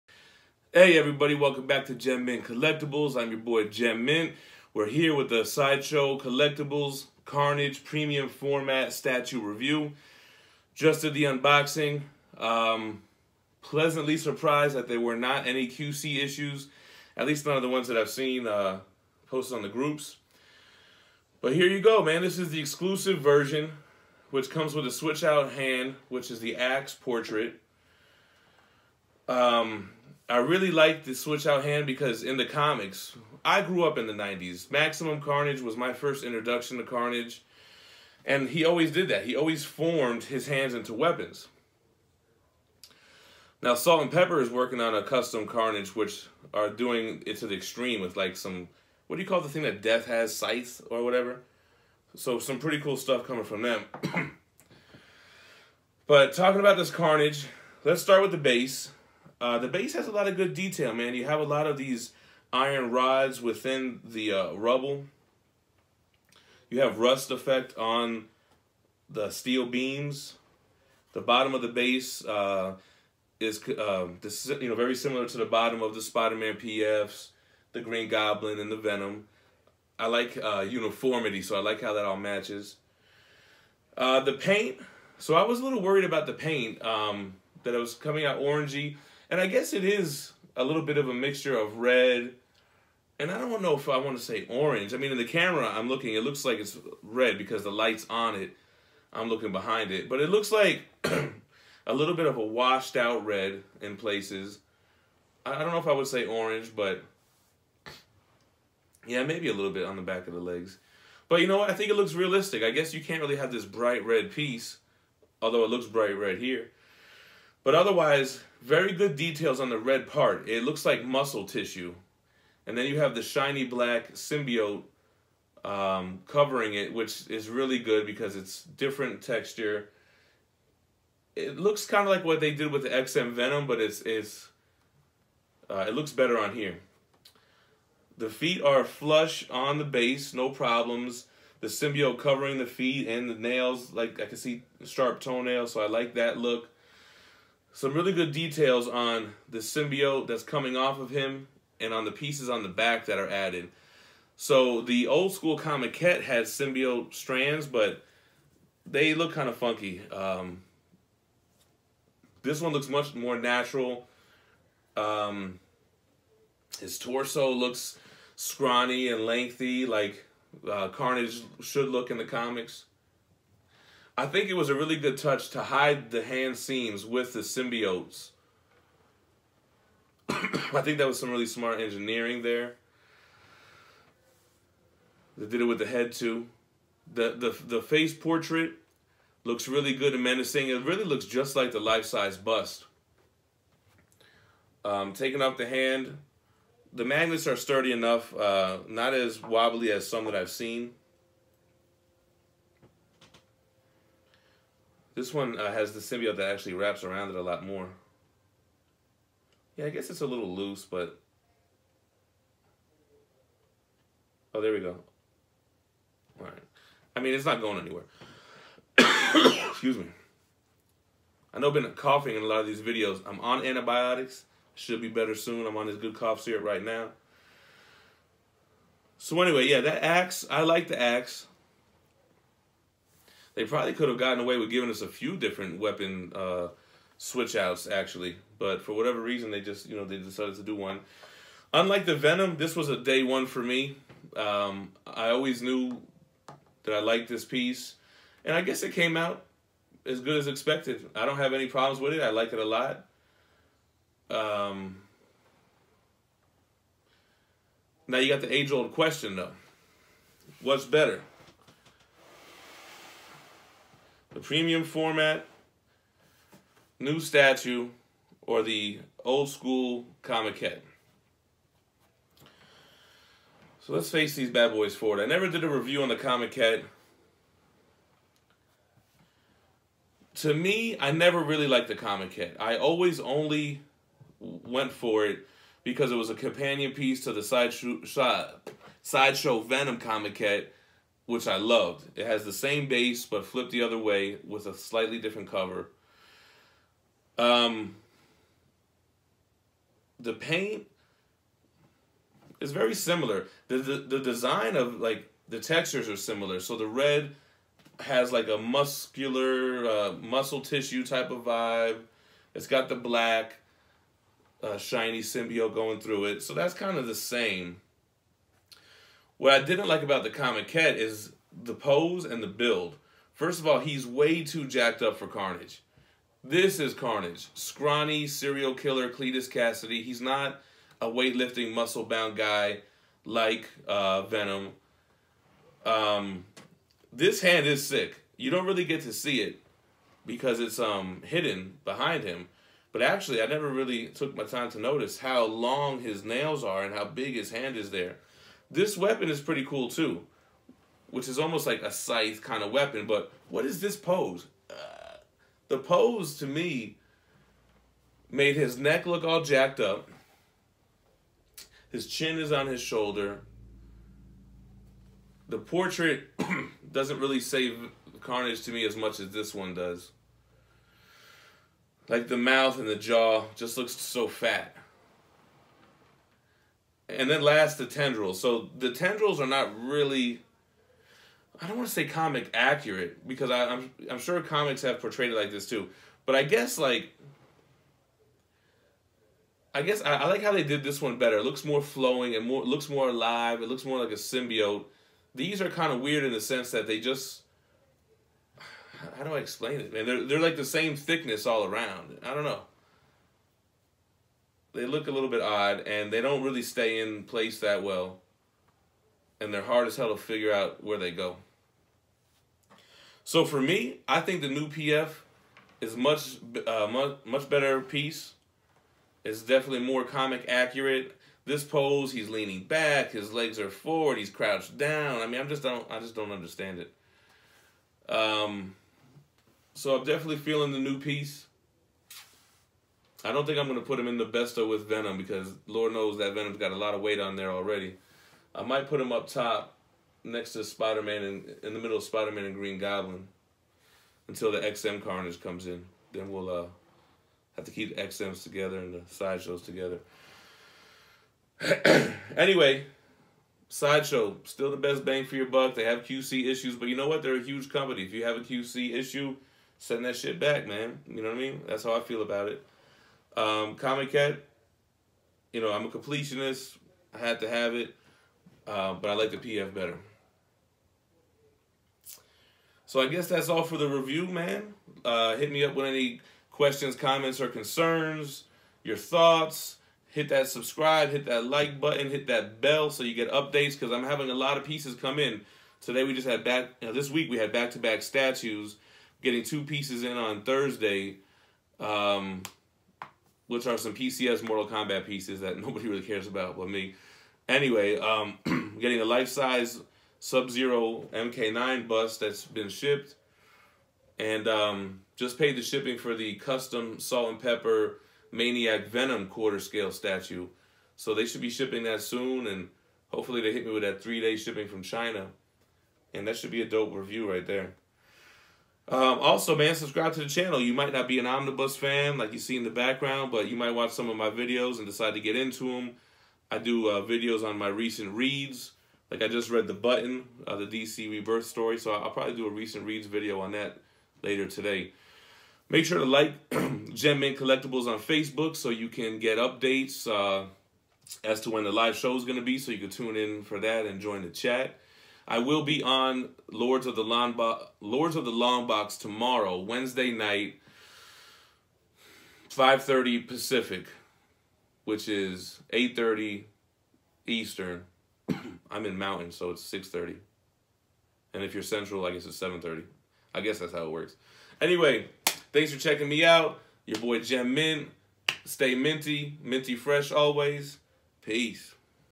Carnage Premium Format by Sideshow Statue Review + Comiquette Comparison
Here’s my review of the new Carnage Premium Format by Sideshow Collectibles. I also bust out the OG comiquette for a comparison.